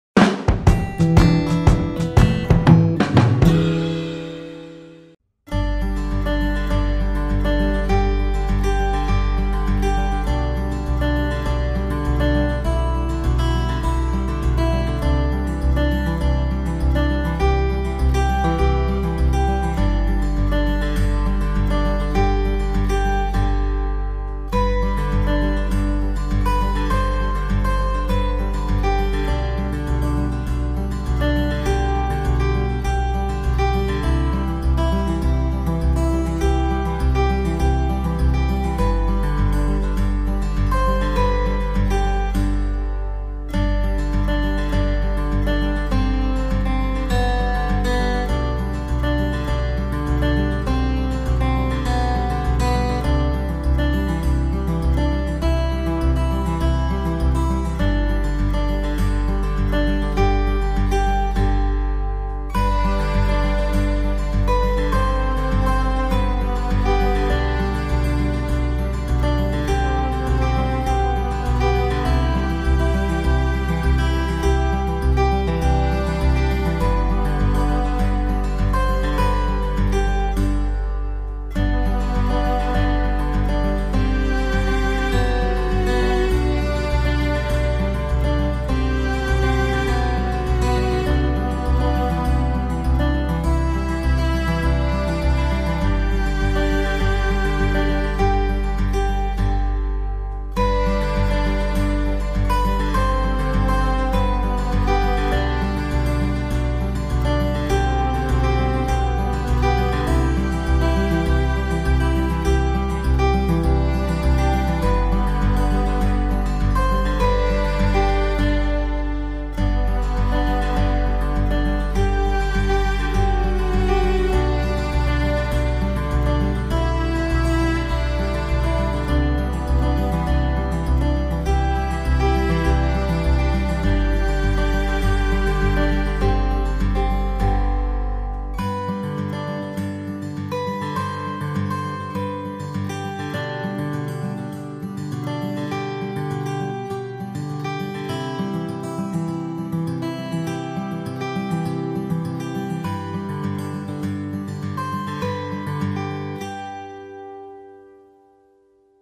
The Pond Drone Video
Here is a video of the beaver pond and trail behind CA Boces in Olean, N.Y. This video was made in April 2020. I hope you enjoy it.